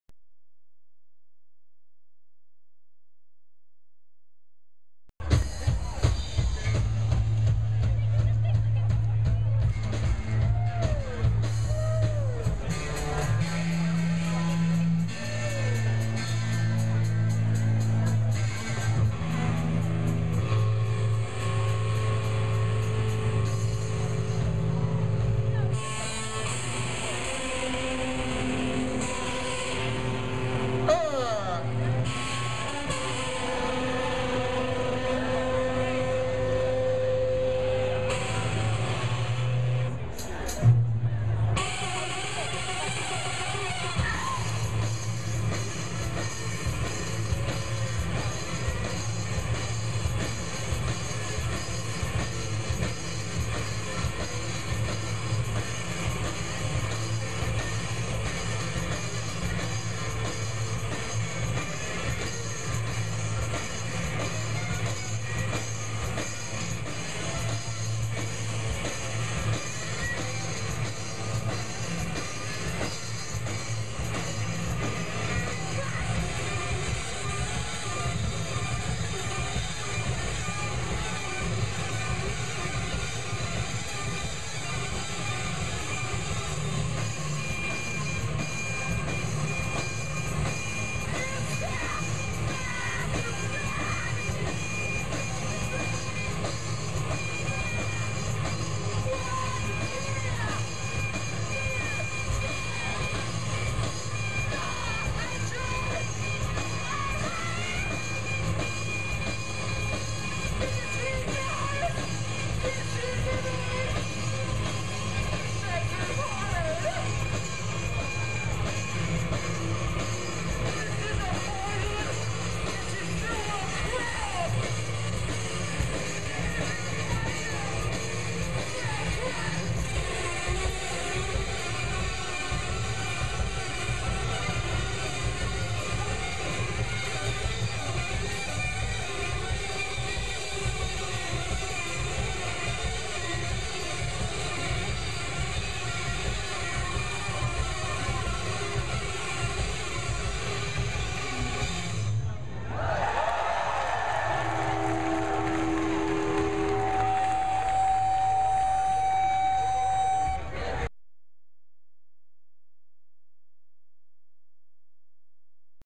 Arab on Radar Whartscape 2010
Arab on Radar at Whartscape July 24th 2010.
 I apologize for how bad this is shot, im trying to do about 8 different things at once, this will get better I promise.